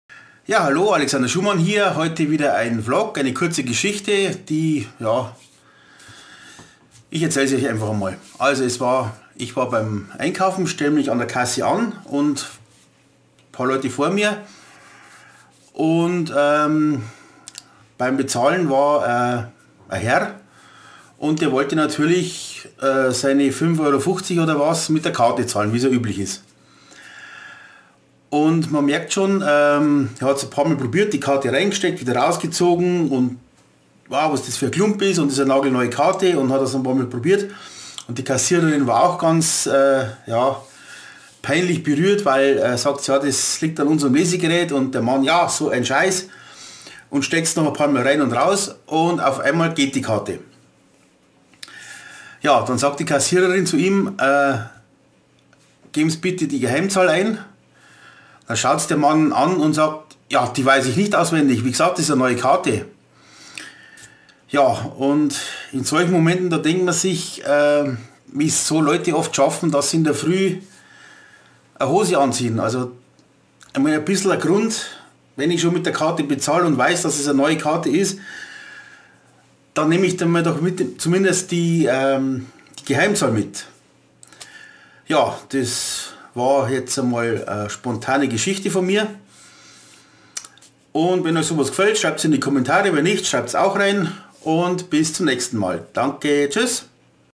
Vlog 11.01.2015 Dumm mit EC Karte
Wenns an der Kasse länger dauert, ist das schlecht. Wenns wegen Idioten länger dauert, dann ist das noch schlechter.